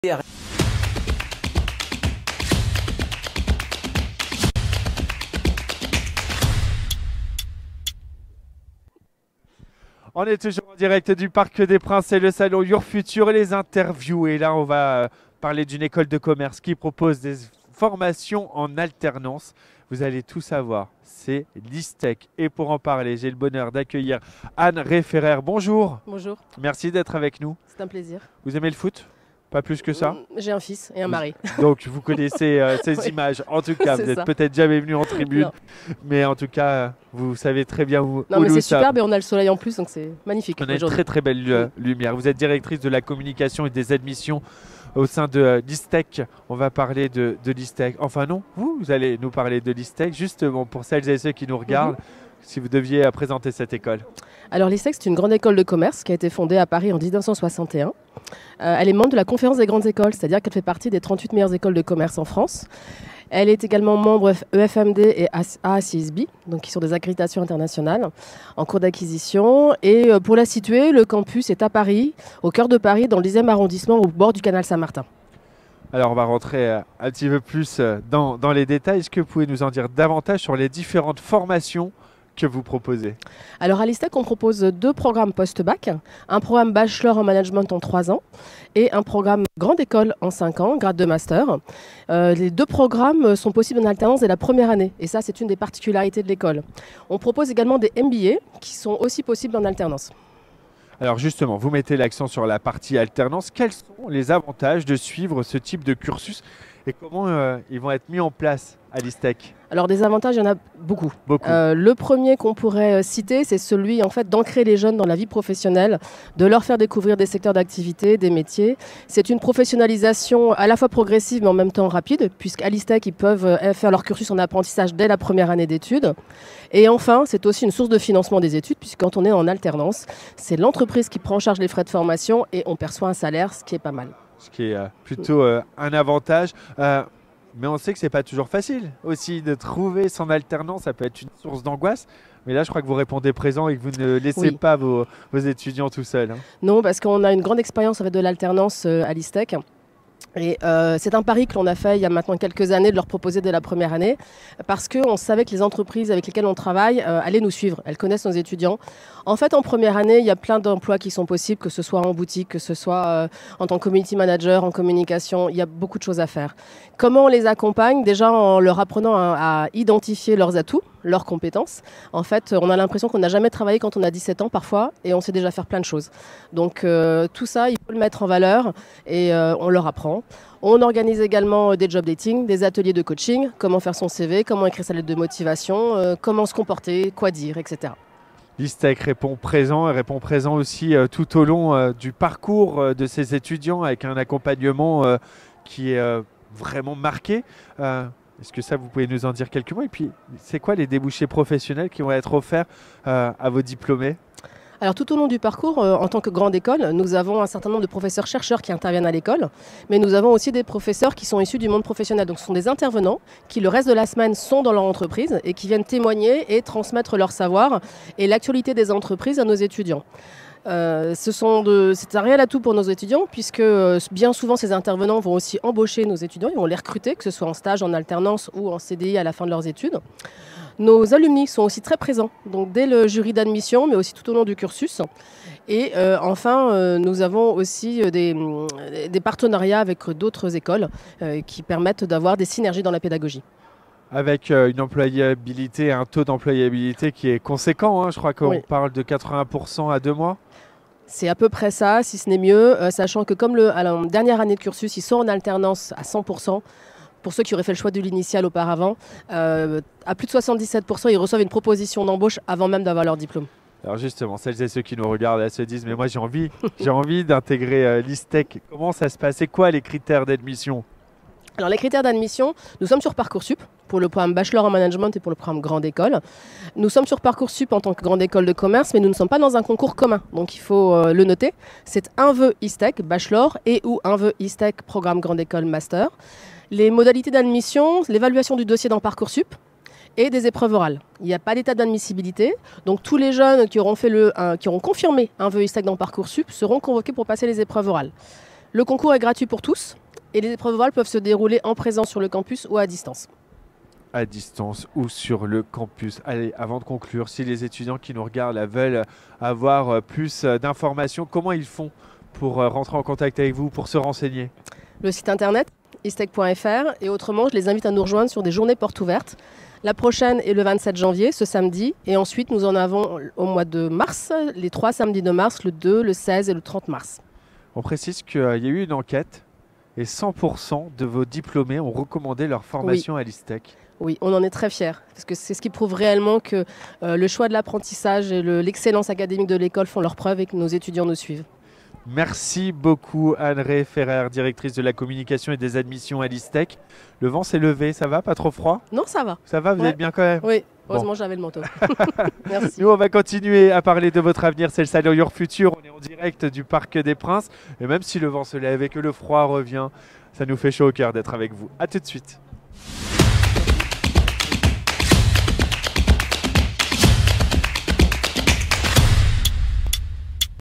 Présentation Istec Business School - Anne Rey Ferrer
Interview de Anne Rey Ferrer, Directrice de la communication et des Admissions de l'Istec Business School.